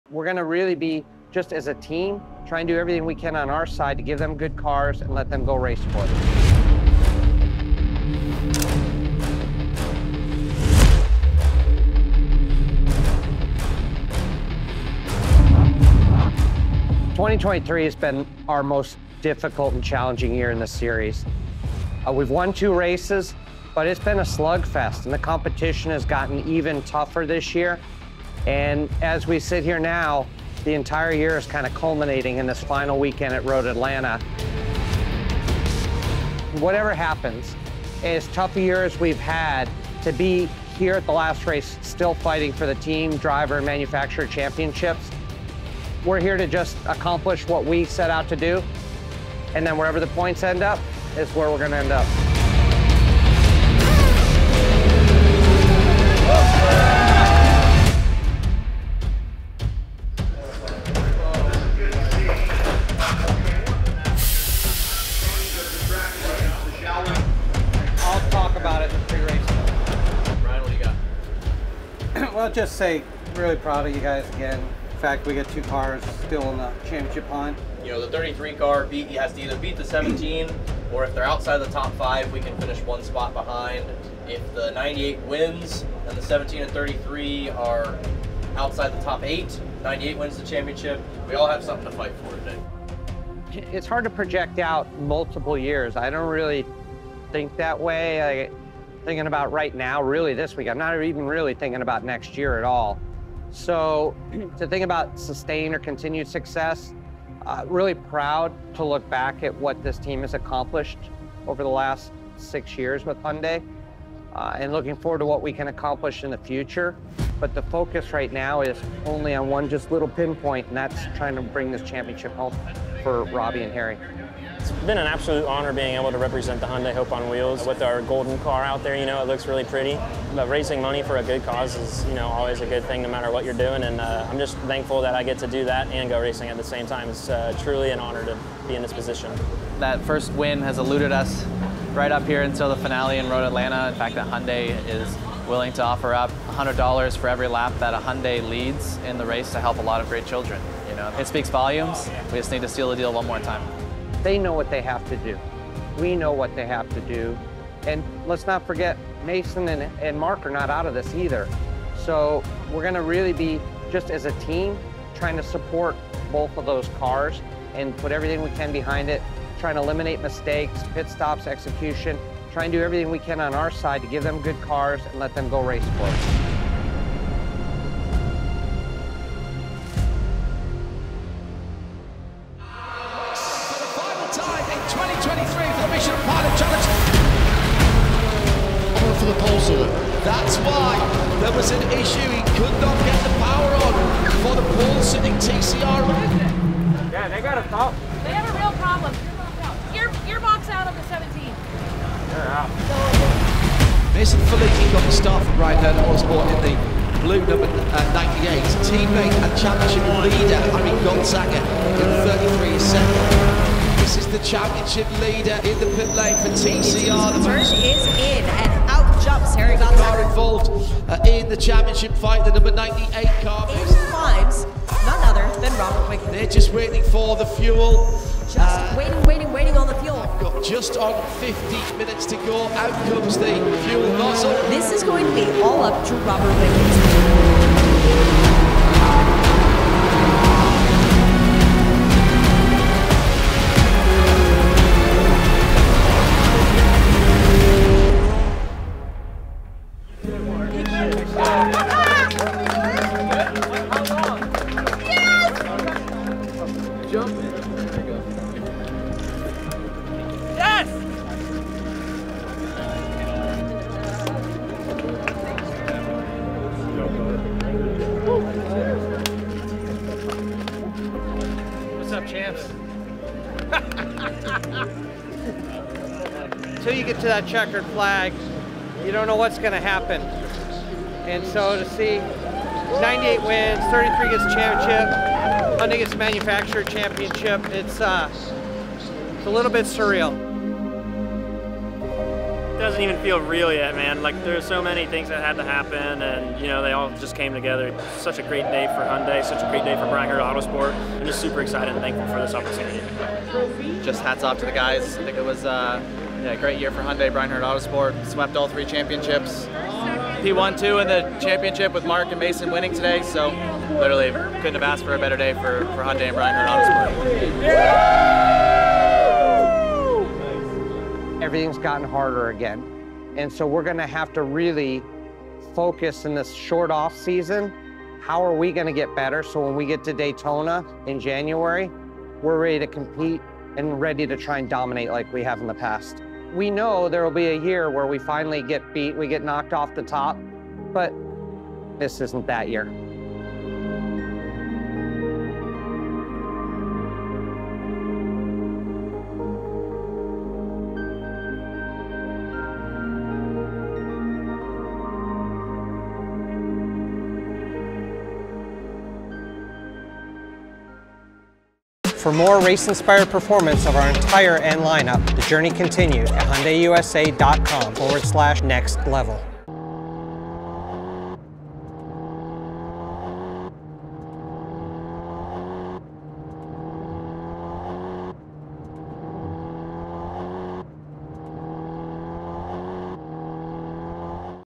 Bringing the Championship Home | Next Level
Coming off one of the most challenging seasons of their careers, team Hyundai and Bryan Herta Auto Sport only had one goal on their mind: Win.

And that’s exactly what they did. The drivers and crew put it all on the line in their ELANTRA N TCRs where Robert Wickens and Harry Gottsacker won the Driver’s Championship in the #33, along with Mason Filippi and Mark Wilkins’ Fox Factory 120 race win in the #98 to secure Hyundai’s Manufacturer’s Title, along with the Team Title win—all to wrap up an incredible 2023 season.